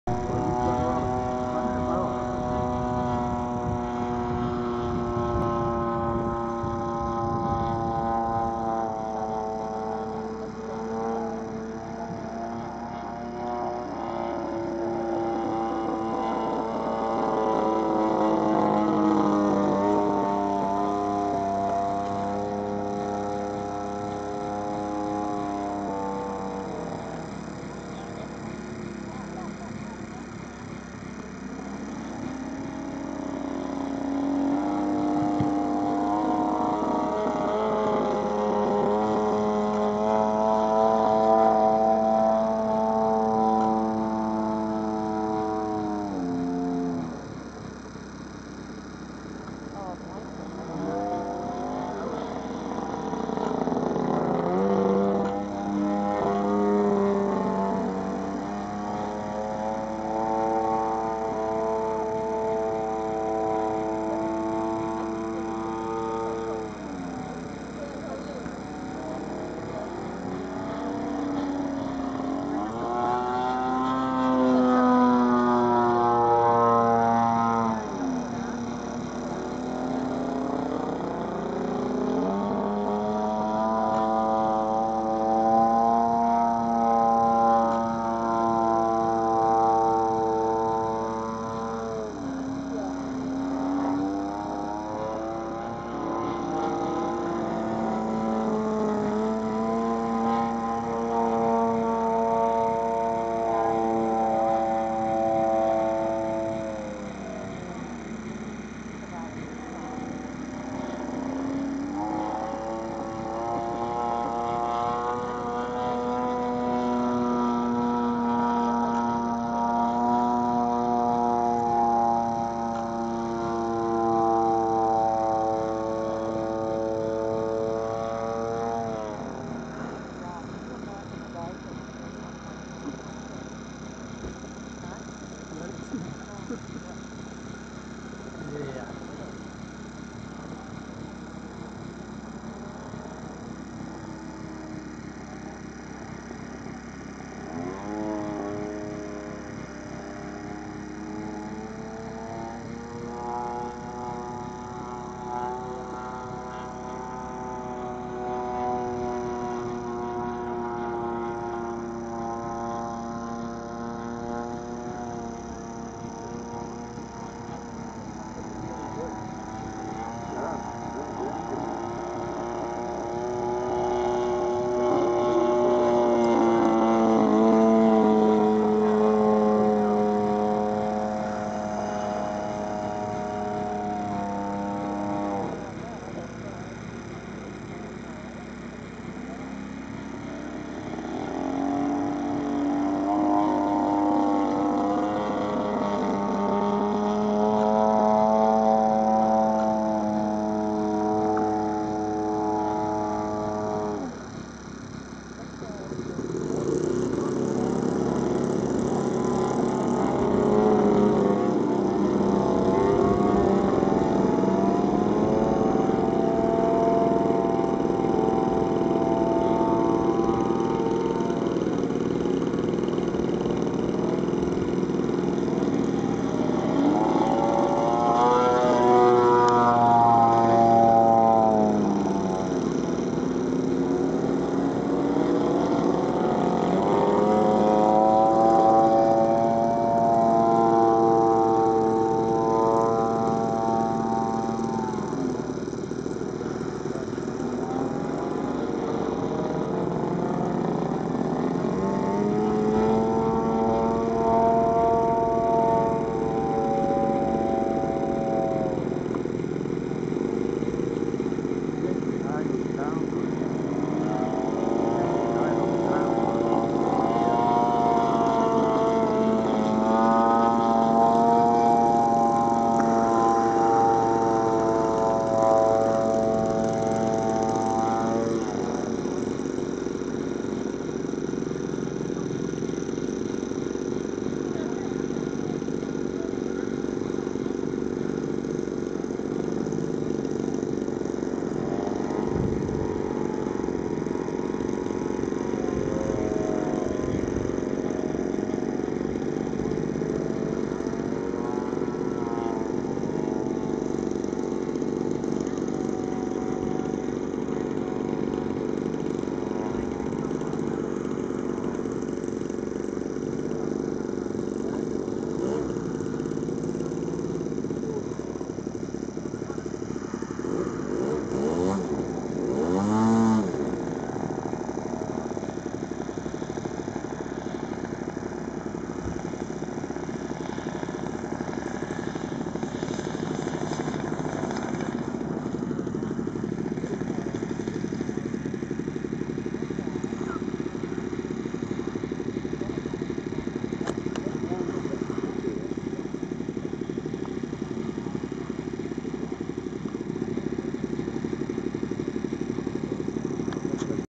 2009 basic IMAC sequence. my first IMAC
2nd round of my first IMAC event. i really rushed through the sequence and it looked pretty bad. great event though. we had a lot of fun. we'll be back for more in 2010.  thanks Neil for the vid